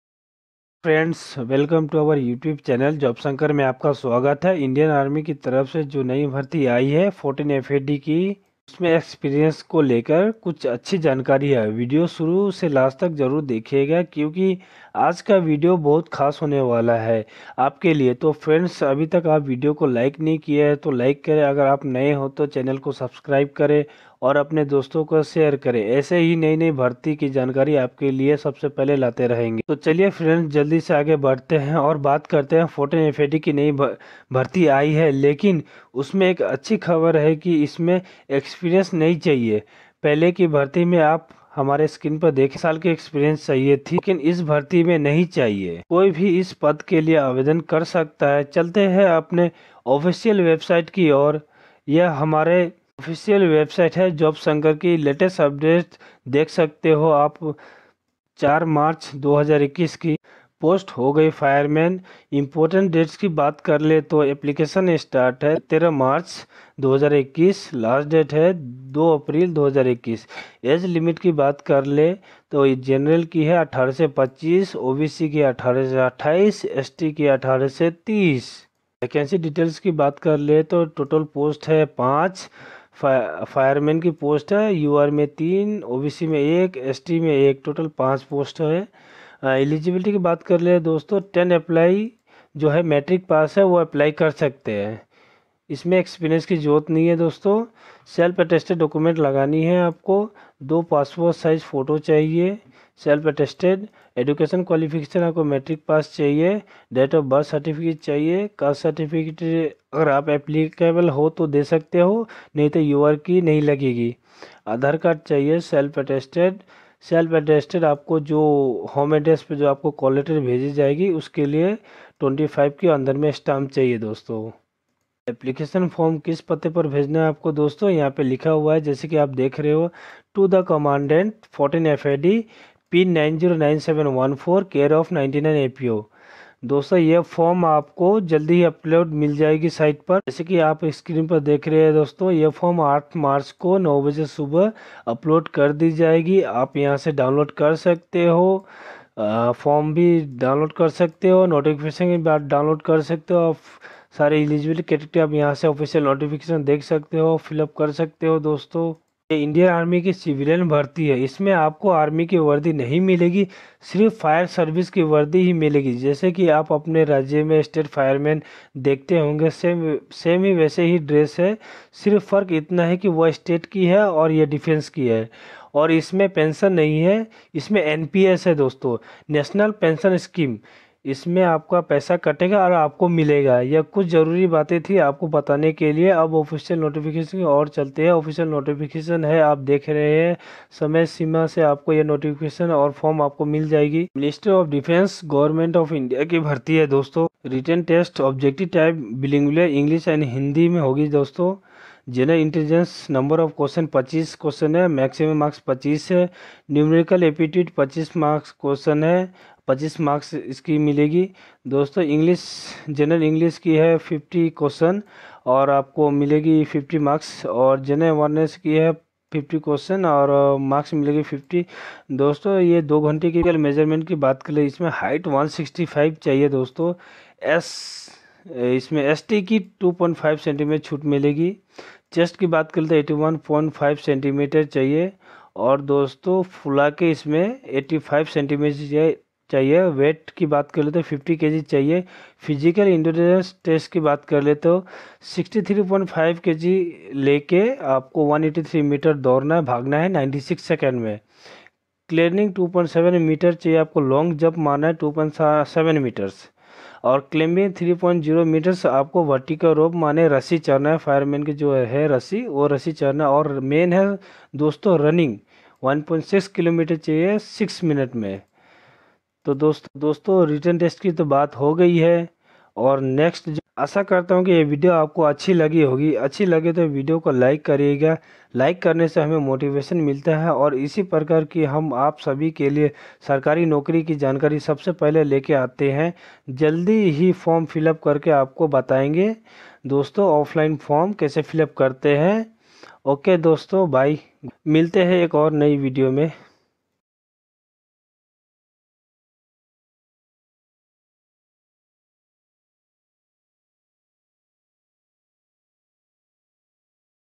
Indian Army 14 FAD Fireman Vacancy 2021 || इंडियन आर्मी फायरमैन पूरी डिटेल्स
14 Field Ammunition Depot Fireman Vacancy 2021 || Indian Army Fireman Civilian Vacancy 2021 ||jobshankar || jobshankar.com || Field Ammunition Depot Guwahati Recruitment 2021